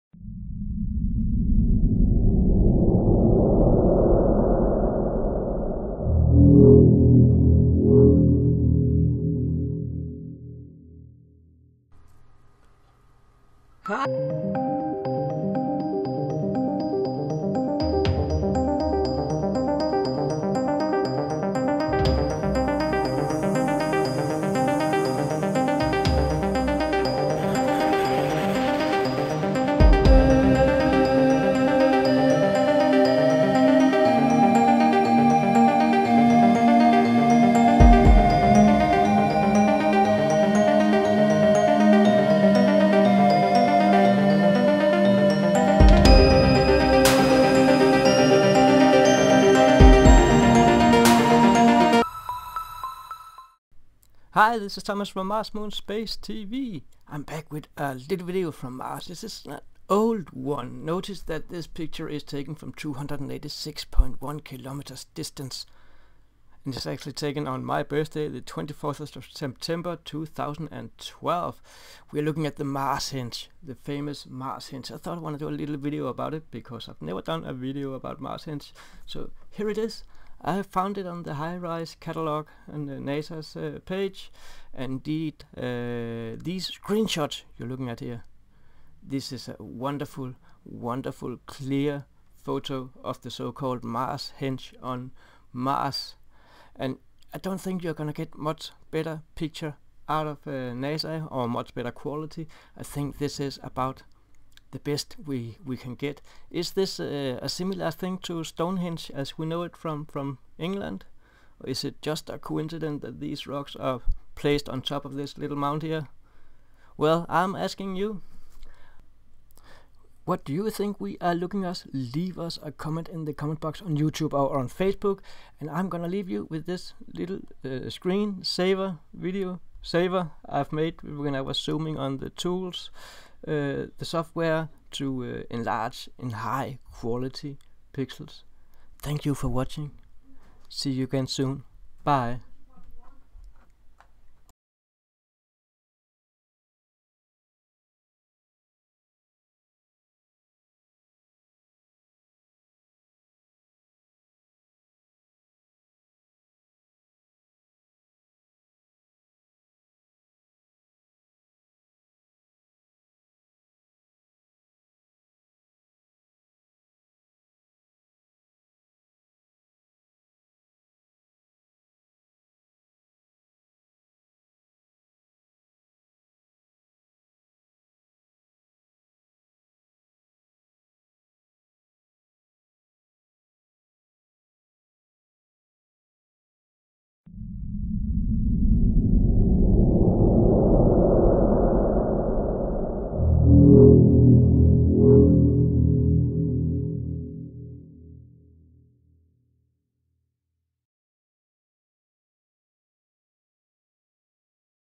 Mars henge High Quality ESP images
Hi Guys.
We deided to have a look back in time and found this image from 2012 that has been featured by many youtubers.
We decided to see how close we could get to the Mars Henge by using NASA's software HIVIEW.